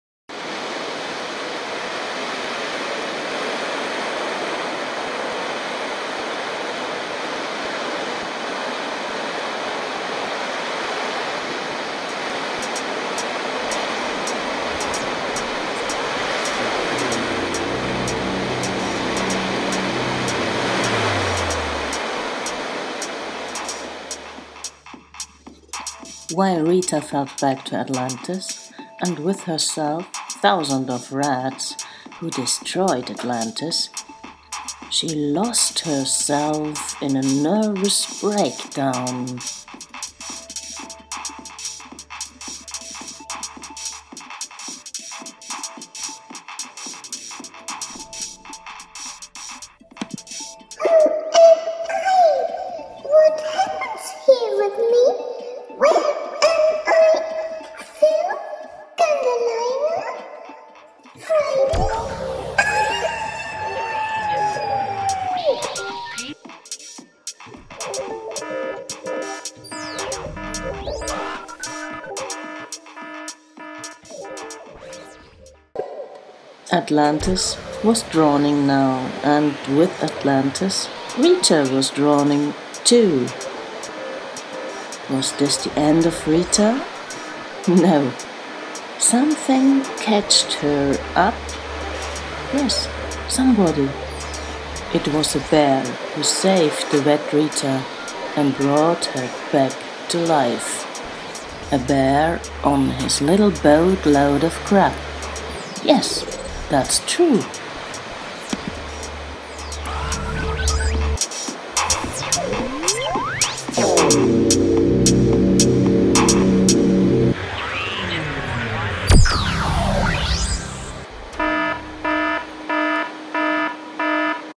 Rita 47 a boat laod of crap
Atlantis was drawning. and Rita? At first yes. but than came a boat load of crap and safed her life.
dedicated to Mr. Bear! (who made lots of songs for Rita)
original chaos music by myself